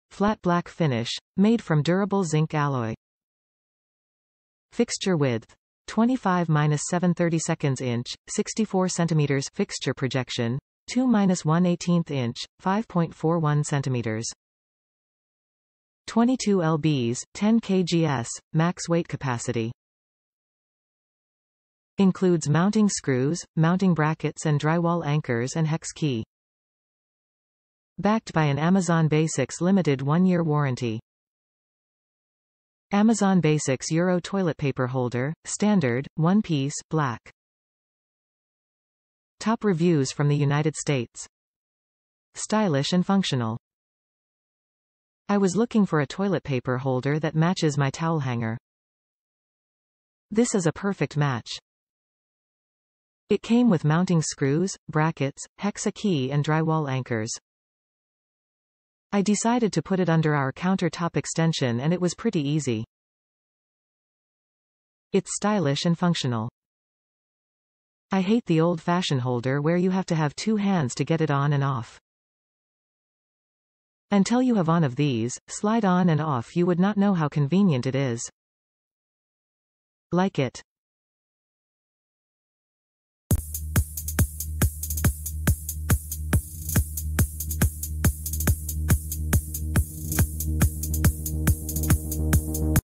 Amazon Basics Euro Toilet Paper Holder - Standard, 1-Piece, Black
Flat Black finish; Made from durable zinc alloy
Fixture Width: 25-7/32-Inch (64 cm) ; Fixture Projection: 2-1/18-Inch (5.41 cm)
22-Lbs (10kgs) max weight capacity
Includes mounting screws, mounting brackets and drywall anchors and hex key
Backed by an Amazon Basics limited 1-year warranty

Stylish and Functional!
I was looking for a toilet paper holder that matches my towel hanger. This is a perfect match. It came with mounting screws, brackets, Hexa key and drywall anchors. I decided to put it under our counter top extension and it was pretty easy. It's stylish and functional. I hate the old fashion holder where you have to have two hands to get it on and off. Until you have on of these, slide on and off you would not know how convenient it is!. Like it!